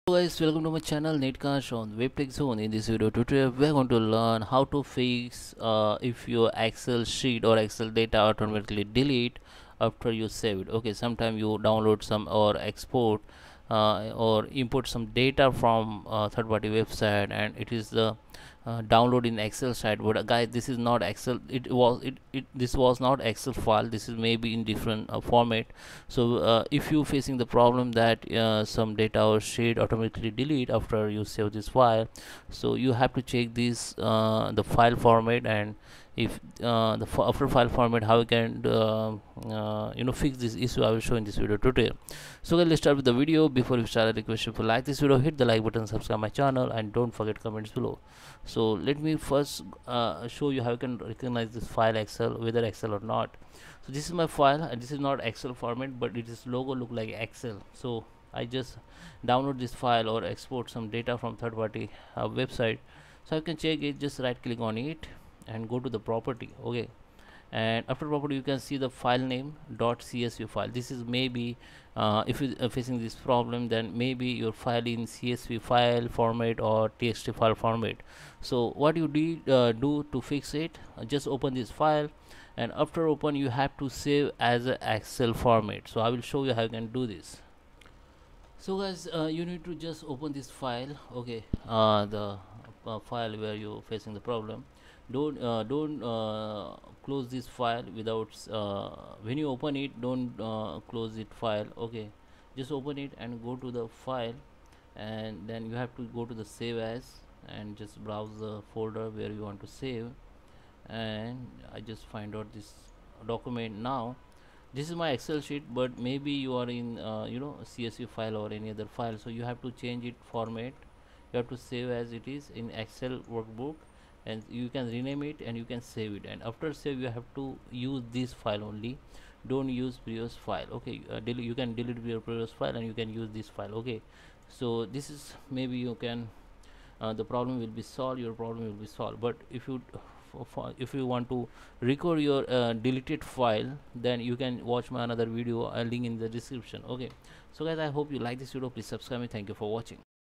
Excel Sheet Automatically Deleted After Save?
In this video tutorial, we will learn to fix, excel sheets that are automatically deleted after saving in excel.